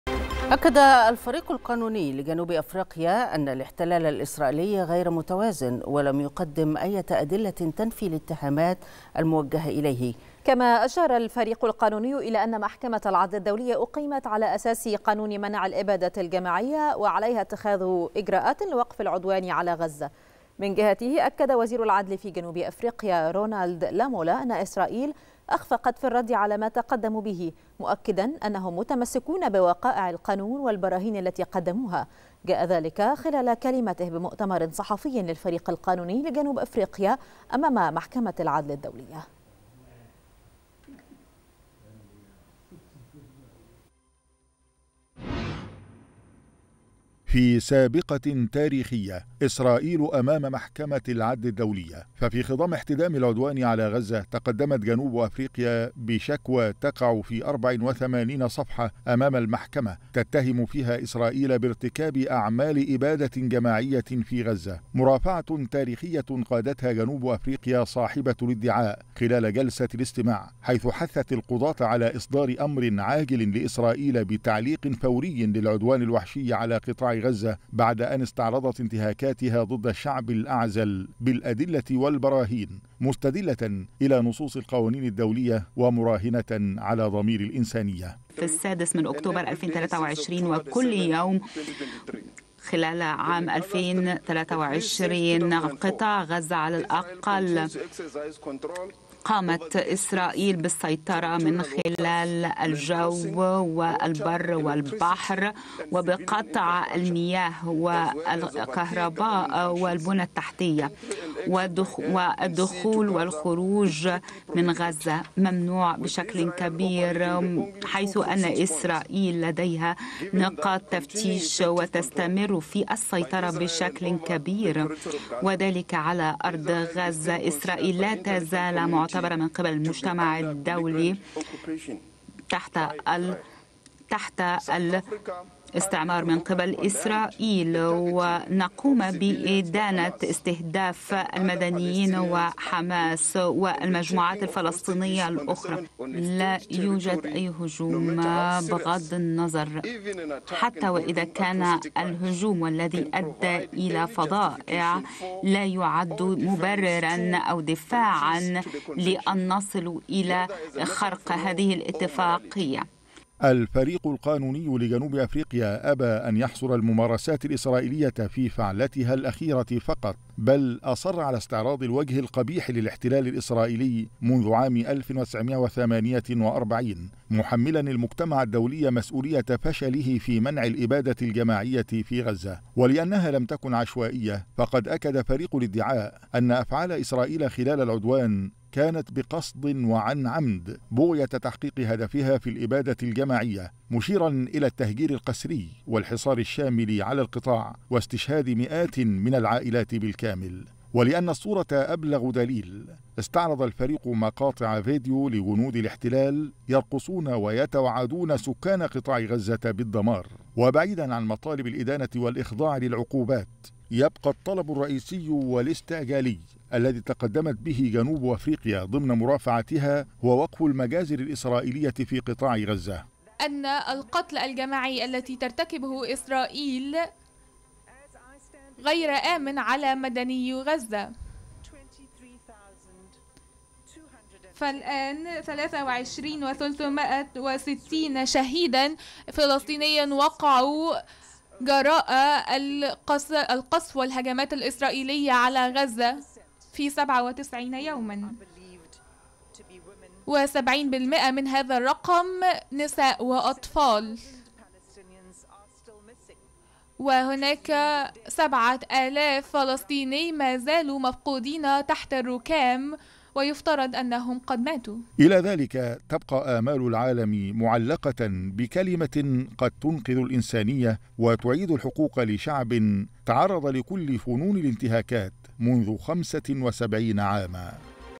الفريق القانوني لجنوب أفريقيا: الاحتلال الإسـ ـرائيلي غير متوازن ولم يقدم أية أدلة تنفي الاتهامات
الفريق القانوني لجنوب أفريقيا: الاحتلال الإسـ ـرائيلي غير متوازن ولم يقدم أية أدلة تنفي الاتهامات الموجهة إليه